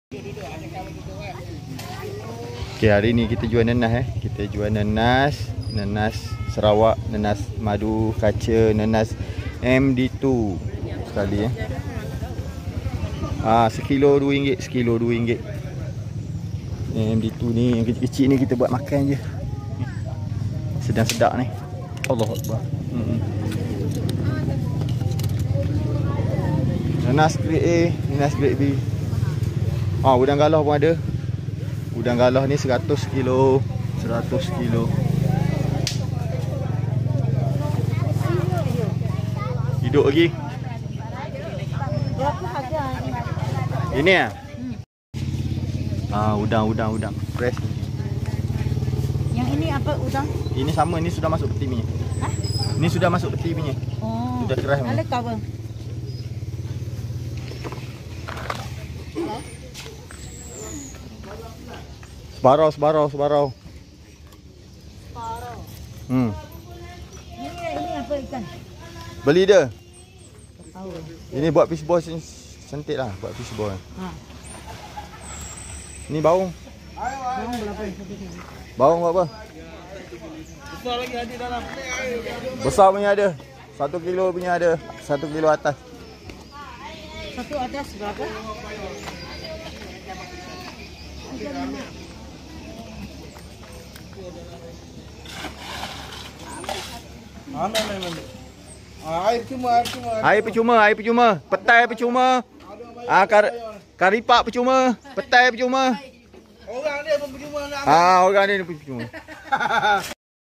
Menu percuma di pekan sari kerayong.
Air dan kuih percuma di pekan sehari kerayong bera.